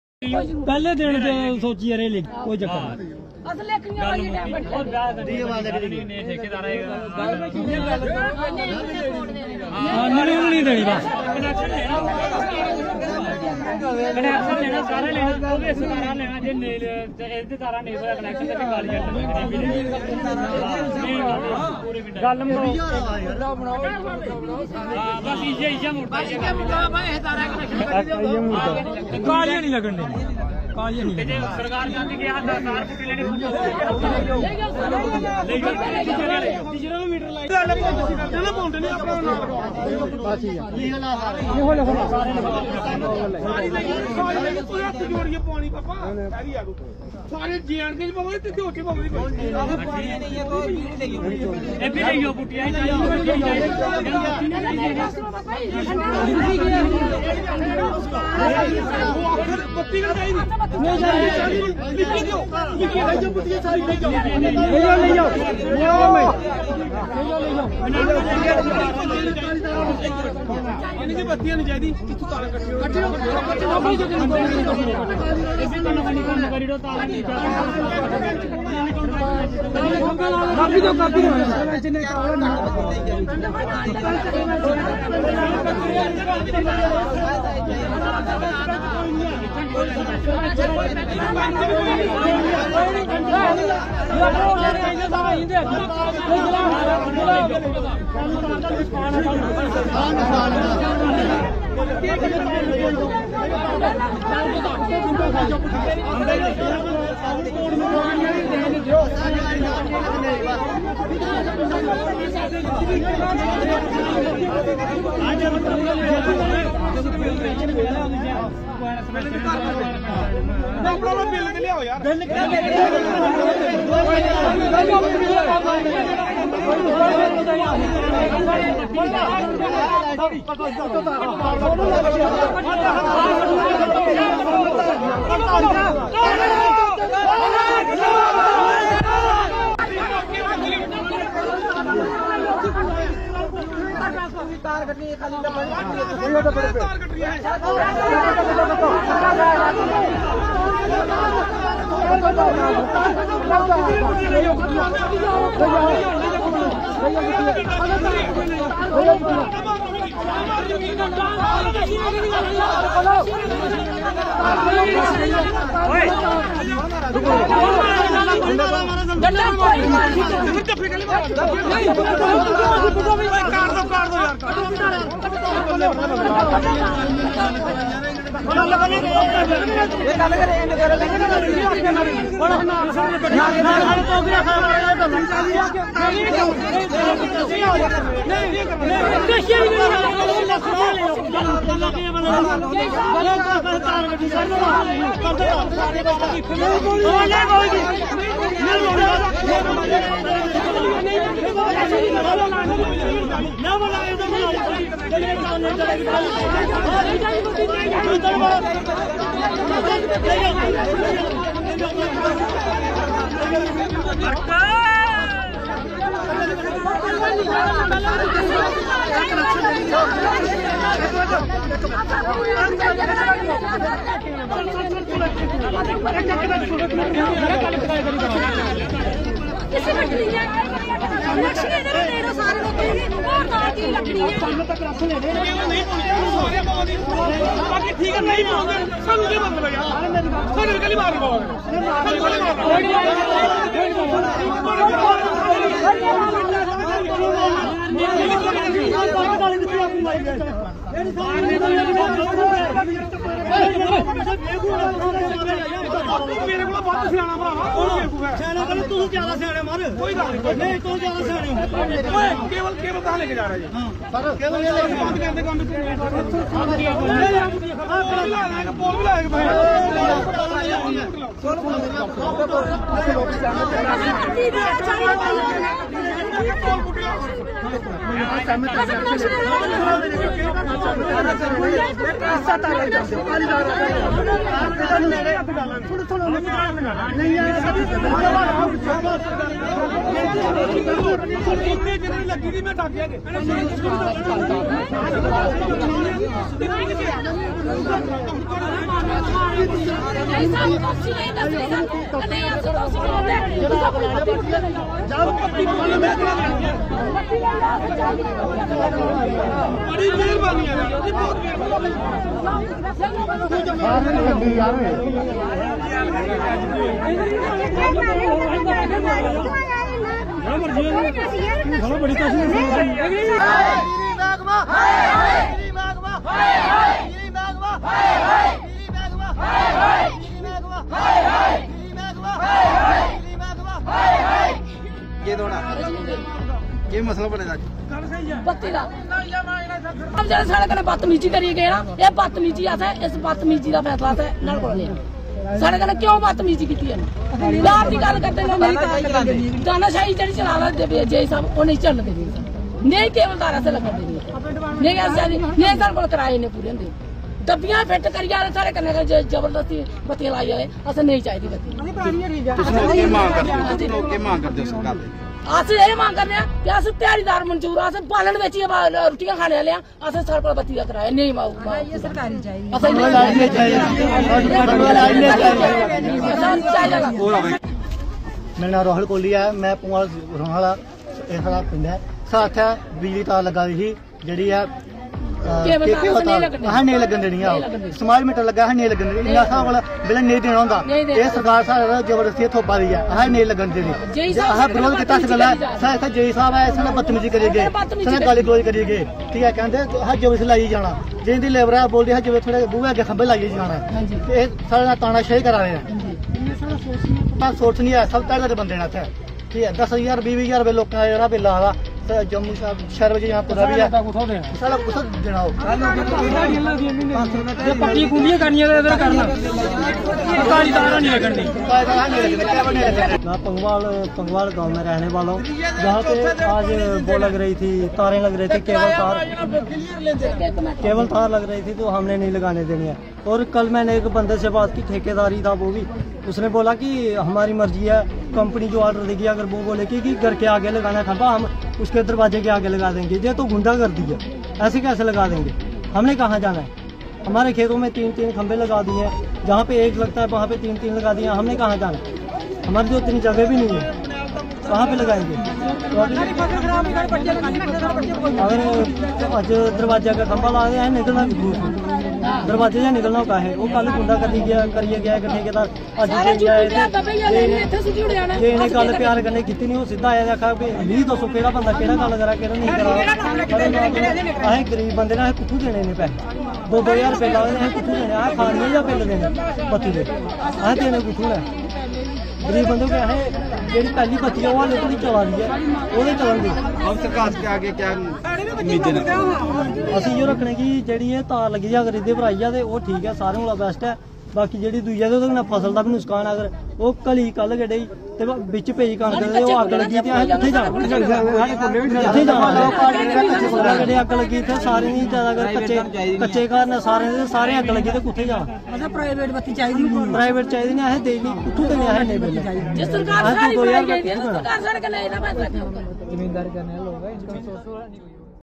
बिजली विभाग के कर्मचारी व गाँव वासियों के बीच तू तू में में।Report by :Pankaj sharma
जिला सांबा के ब्लॉक सुंब की पंचायत समलाह के गाँव pangwal मे इलेक्ट्रिसिटी केबल वायर को न लगाए जाने पर बिजली विभाग के कर्मचारी व गाँव वासियों के बीच तू तू में में।
Report by :Pankaj sharma